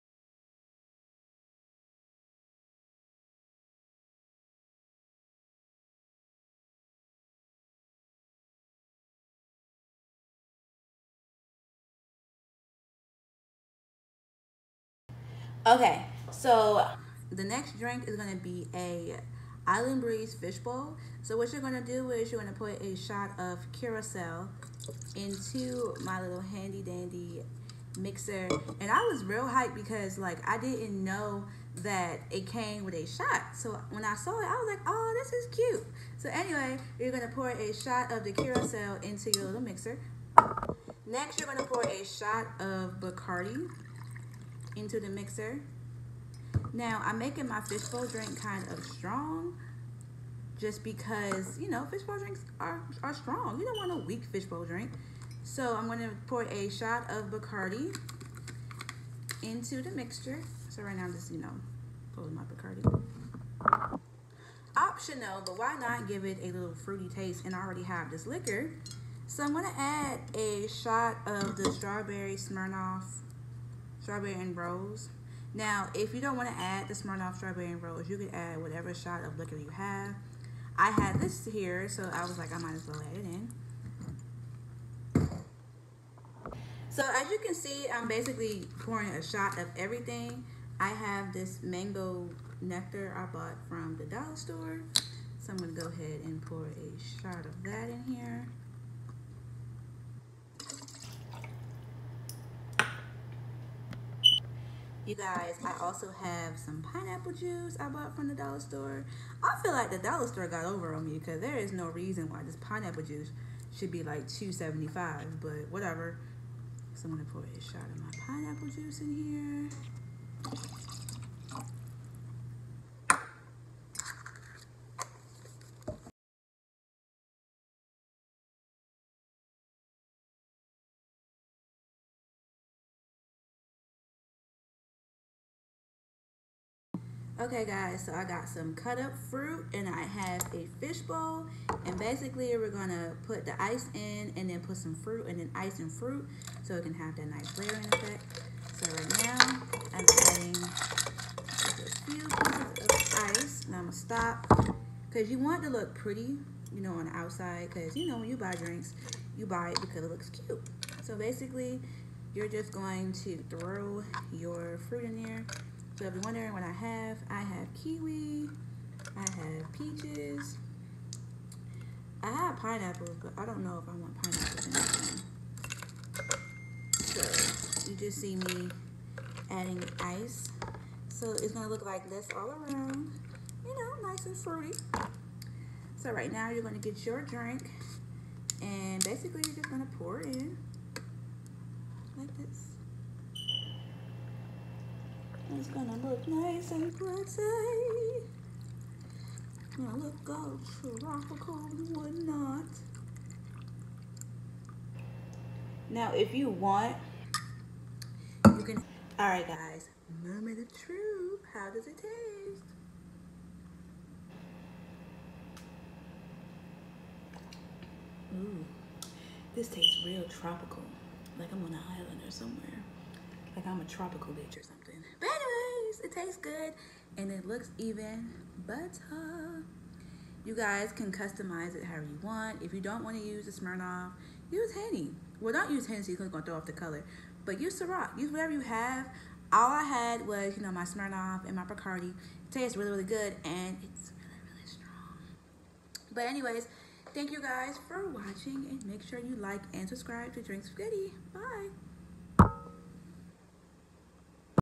Bacardi Strawberry Island Breeze Fish Bowl Tutorial
BACARDI ISLAND BREEZE FISH BOWL DRINK

WANT MORE DRINK RECIPES
CLICK HERE TO BE APART OF MY PRIVATE DRINK COMMUNITY
MEMBERS WILL HAVE UNLIMITED ACCESS TO NEVER BEFORE SEEN DRINK TUTORIALS AND WILL RECEIVE SPECIAL DISCOUNTS ON ALL goodiebagsbylee.com products

WHAT TO BUY
Pineapple Juice
Blue Curacao
Mango Nectar
Assorted Fruit
Bacardi
glass fish bowl

Pour
1 shot of Bacardi
1 shot of Blue Curacao
1 shot of Mango Nectar
1 shot of vodka into shaker

SHAKE
pour over ice garnish with fruit

LETS TALK

Business Instagram @goodiebagsbylee
Personal Instagram @_meangirllee